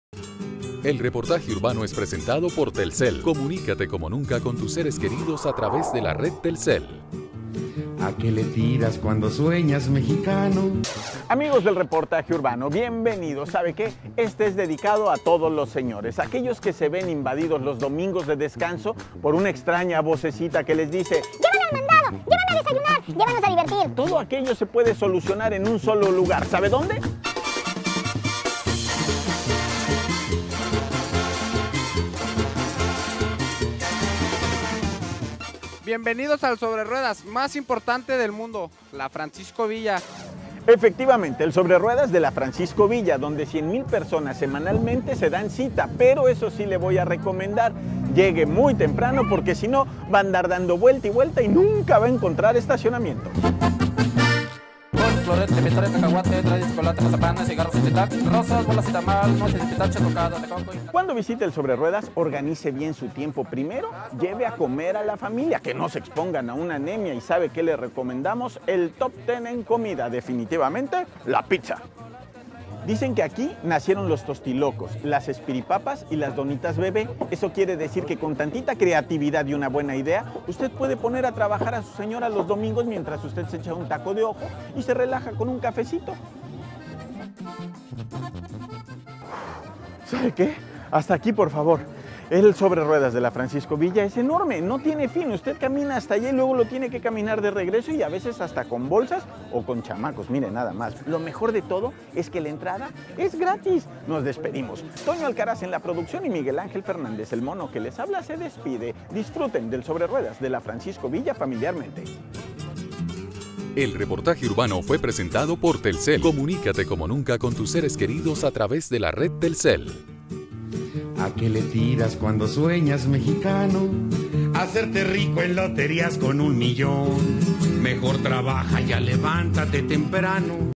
Reportaje Urbano "Sobreruedas"
Visitamos el sobre ruedas mas grande de Tijuana.